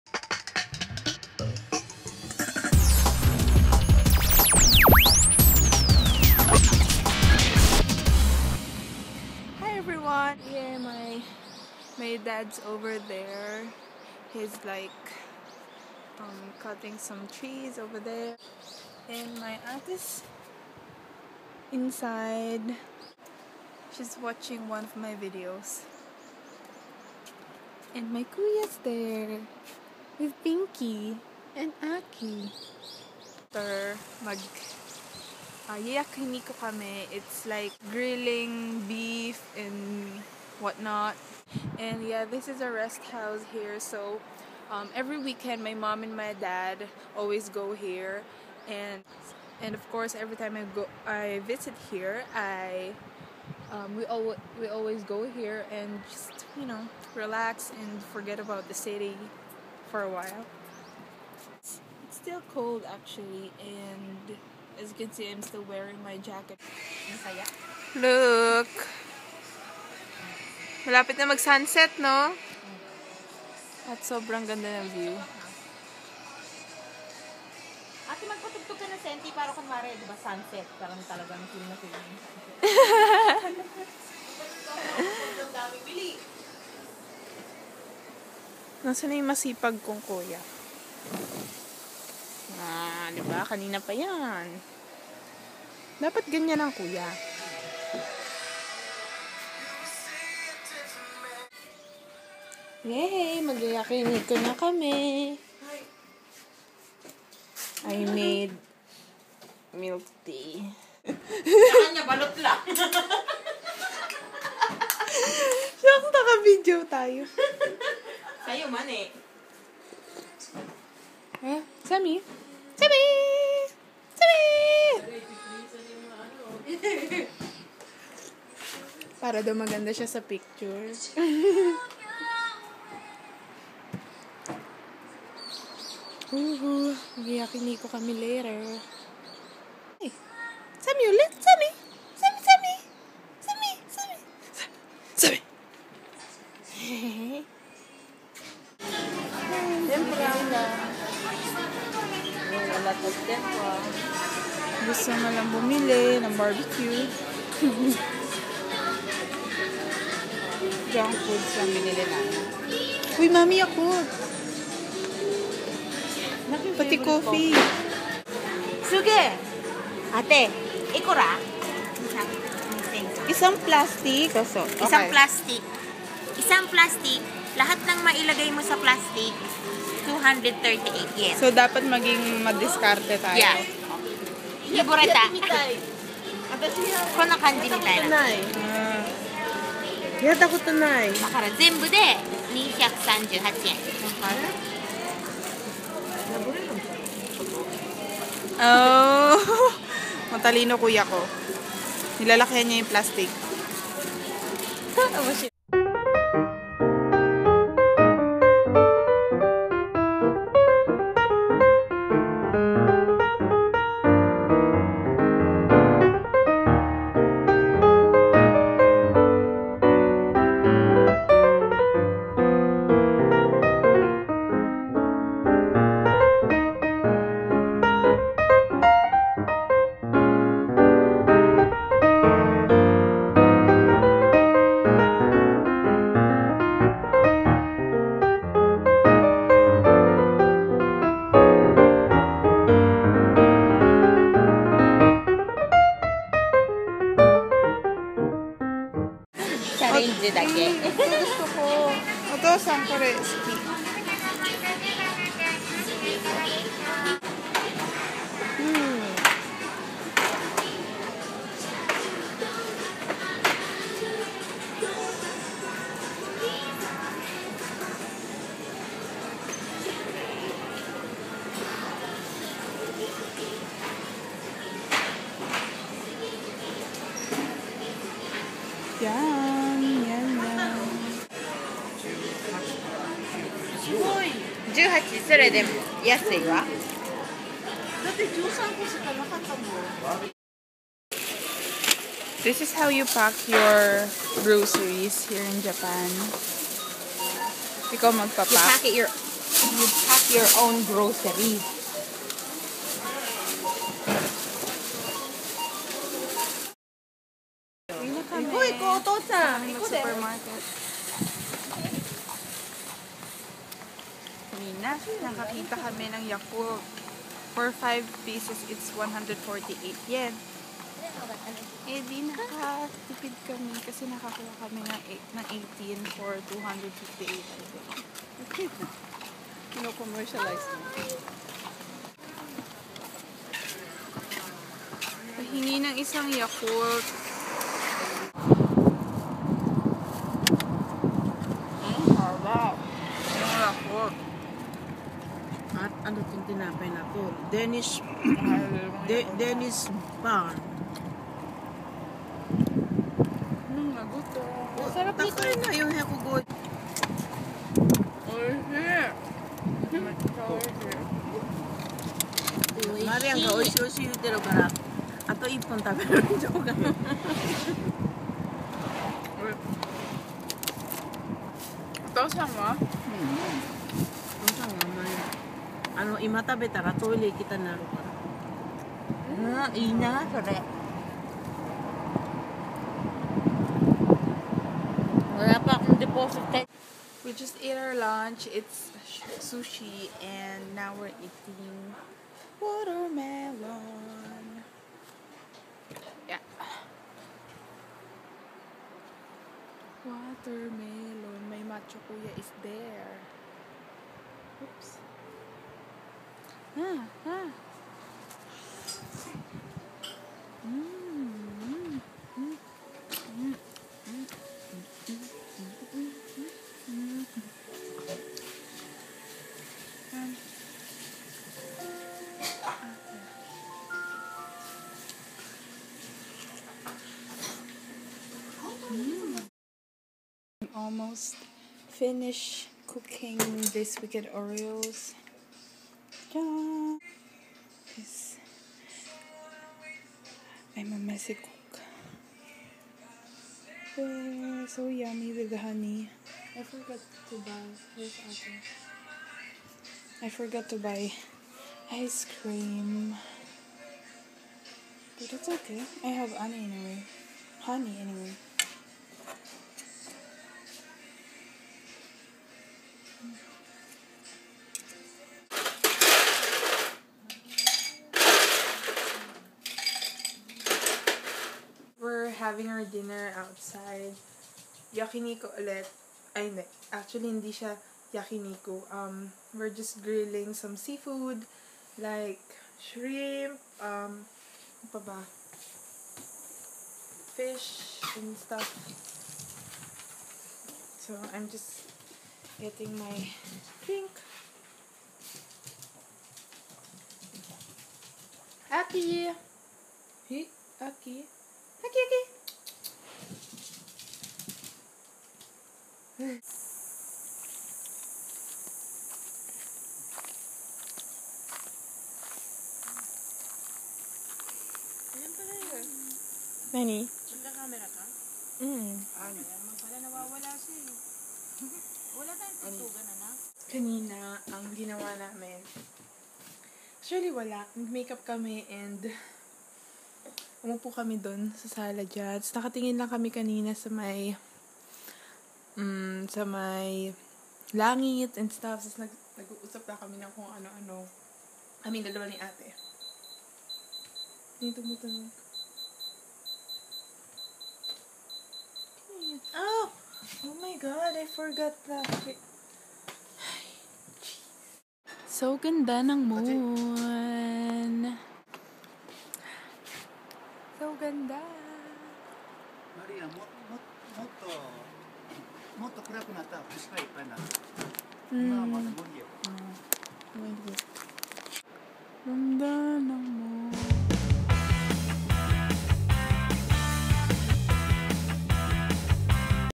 YAKULT ALL YOU WANT! - May 4 & 5, 2012 - The Mayan Calendar
Heeey!!

So I've visited our beautiful rest house in Wakayama, Japan with my lovely family. Just 1 and half to 2 hour drive from our city Osaka.

xoxo Mayan